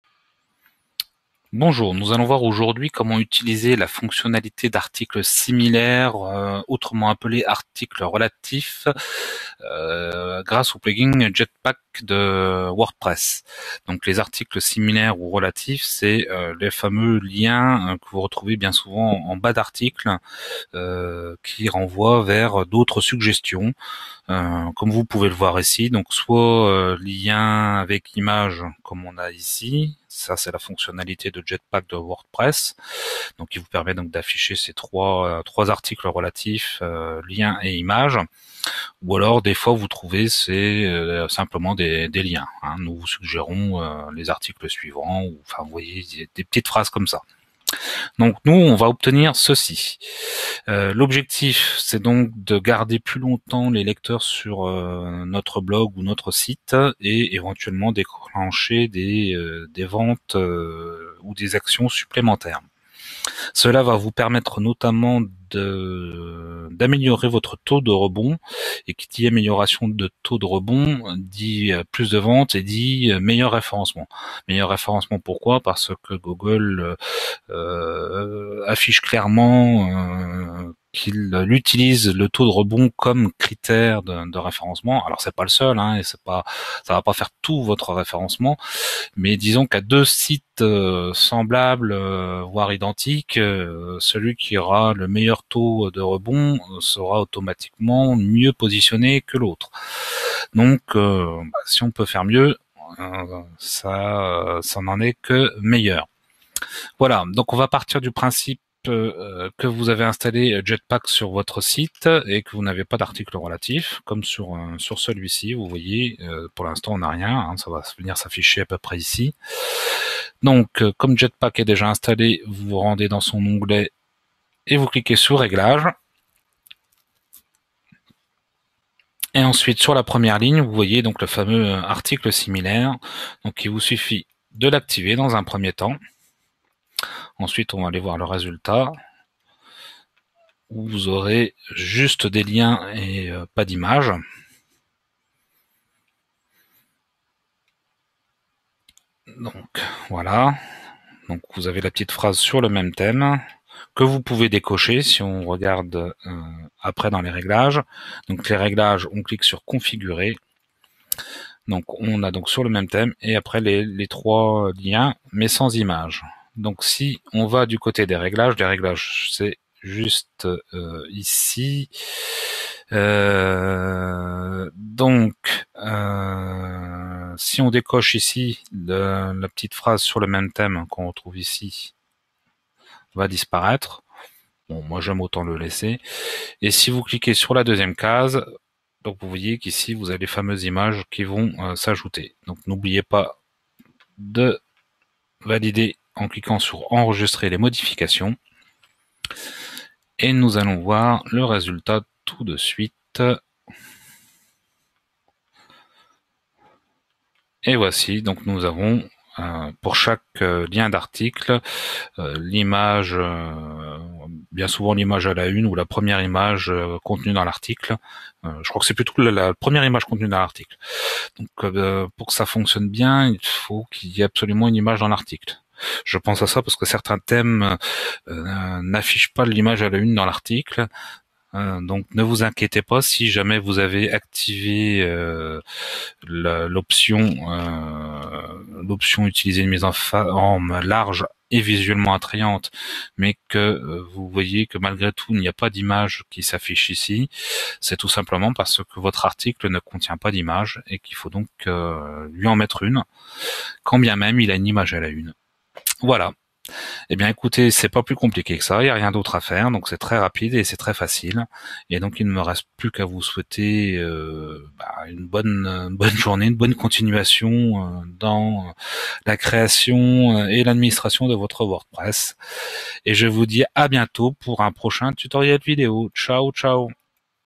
Activer et paramétrer les articles similaires via JetPack pour WordPress
La plateforme gratuite vous propose de vous montrer comment activer et paramétrer les articles relatifs grâce à JetPack pour WordPress.
Si vous avez besoin d'aide ou d'un prestataire concernant WordPress, contactez